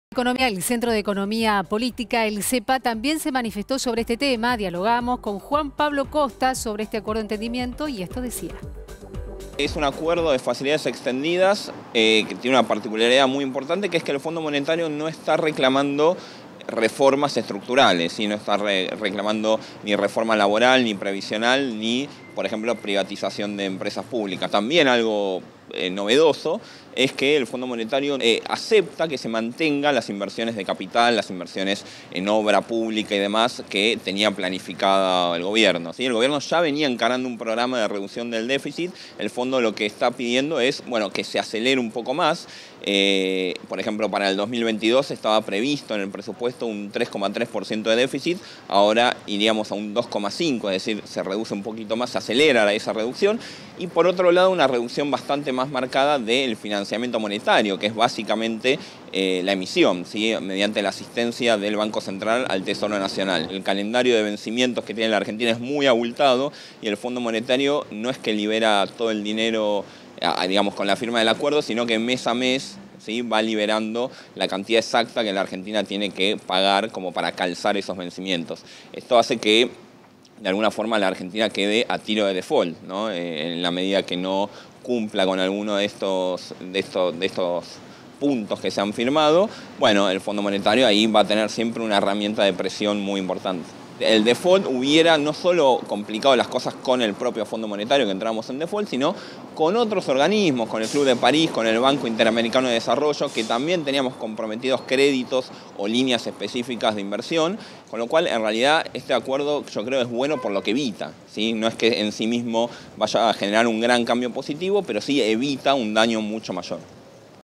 Para un especialista, "el acuerdo evita un daño mucho mayor"
Juan Pablo Acosta, economista e integrante del Centro de Economía Política Argentina (CEPA) se refirió a la negociación con el FMI y aseguró que "en un escenario complejo" es el acuerdo “menos malo” que se logró.